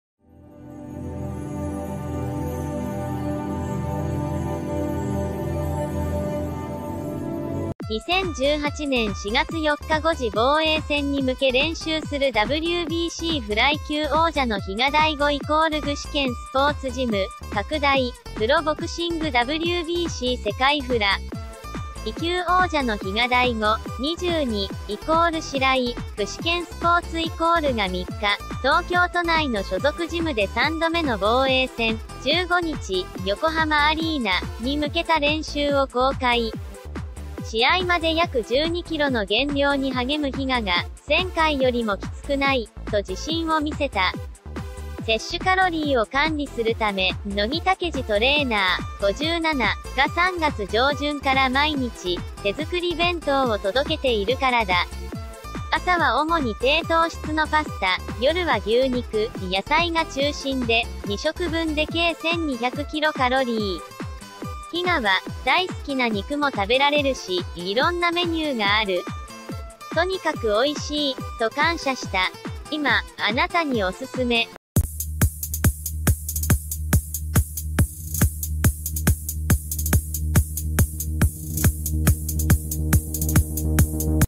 最新ニュース| 比嘉、減量１２キロは余裕！？／ＢＯＸ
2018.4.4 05:00 防衛戦に向け練習するＷＢＣフライ級王者の比嘉大吾＝具志堅スポーツジム【拡大】 　プロボクシングＷＢＣ世界フライ級王者の比嘉大吾（２２）＝白井・具志堅スポーツ＝が３日、東京都内の所属ジムで３度目の防衛戦（１５日、横浜アリーナ）に向けた練習を公開。試合まで約１２キロの減量に励む比嘉が「前回よりもきつくない」と自信を見せた。摂取カロリーを管理するため、野木丈司トレーナー（５７）が３月上旬から毎日、手作り弁当を届けているからだ。朝は主に低糖質のパスタ、夜は牛肉、野菜が中心で、２食分で計１２００キロカロリー。比嘉は「大好きな肉も食べられるし、いろんなメニューがある。とにかくおいしい」と感謝した。 今、あなたにオススメ